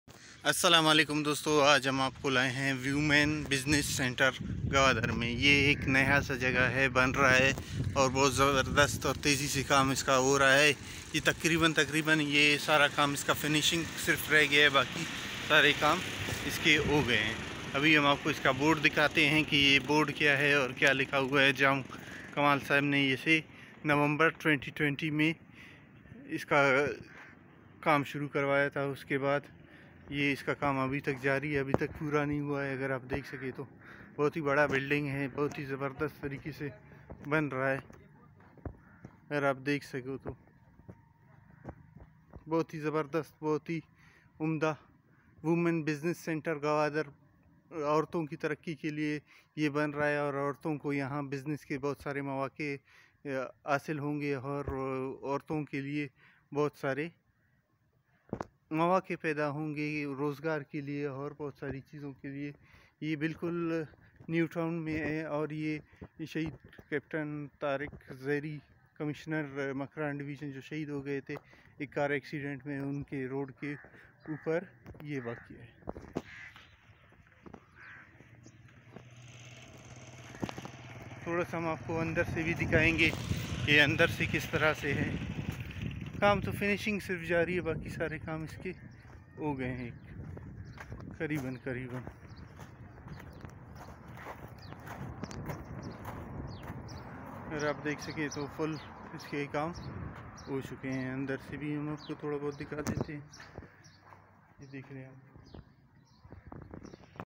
Women Business Center Gwadar
women business center is being built in gwadar new town phase 1. here is the place were women can work easily. its help gwadar and pakistan growing. woman can show their talent to the world what can they do for the economy of pakistan. very nice and appreciating.